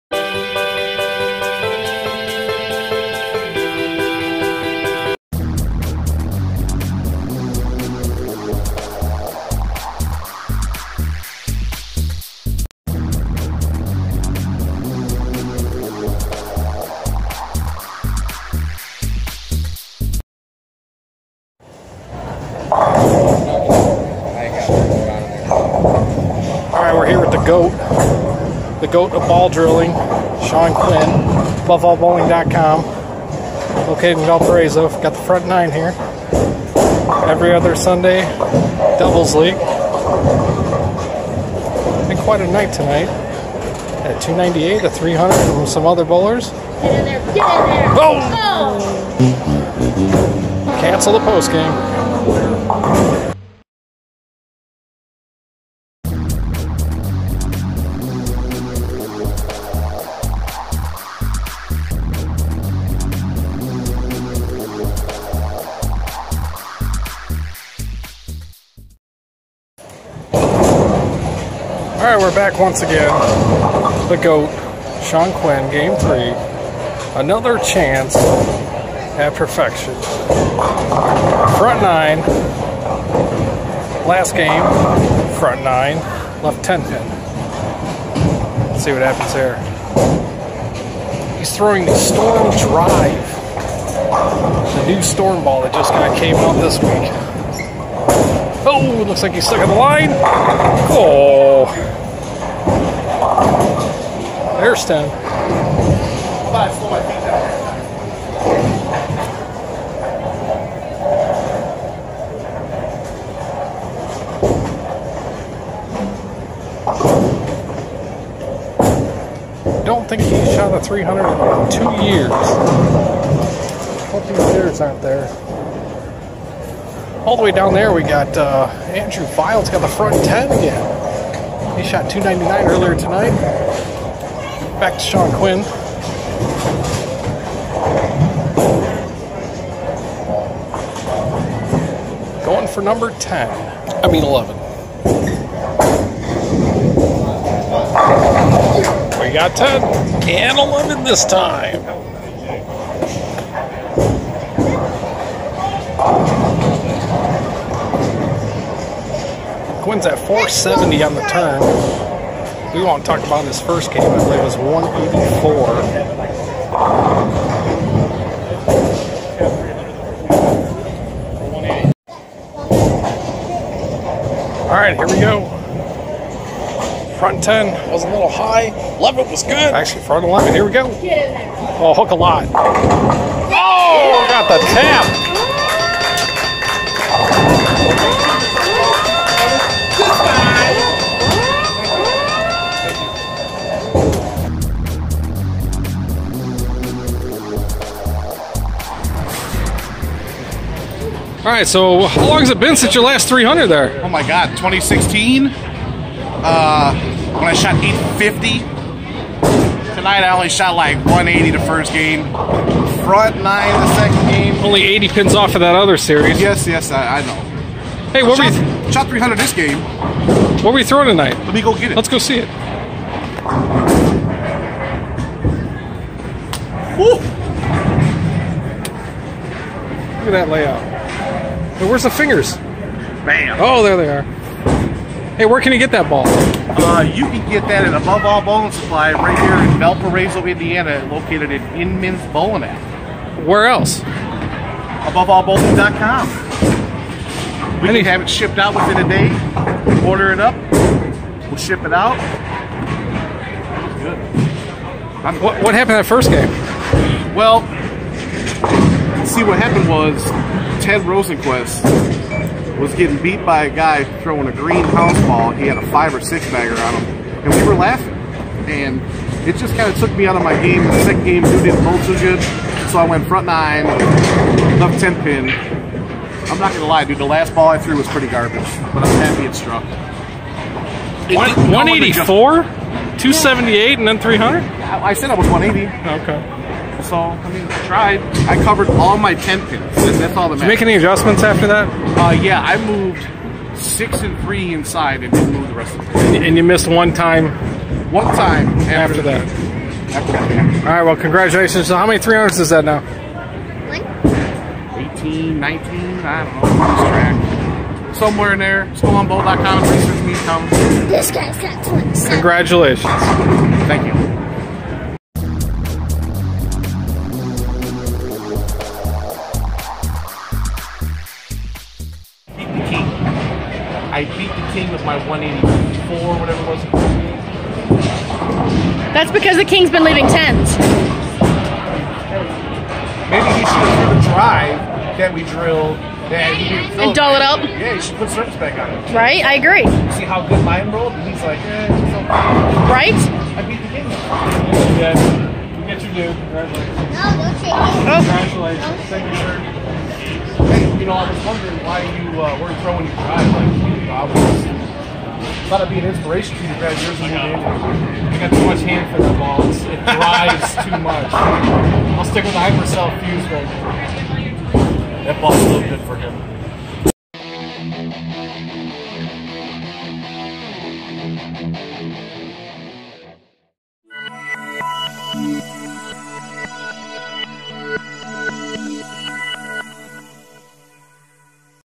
2/11/18 Shaun Quinn 300 game took 2 tries
From the Every Other Week Sunday Doubles League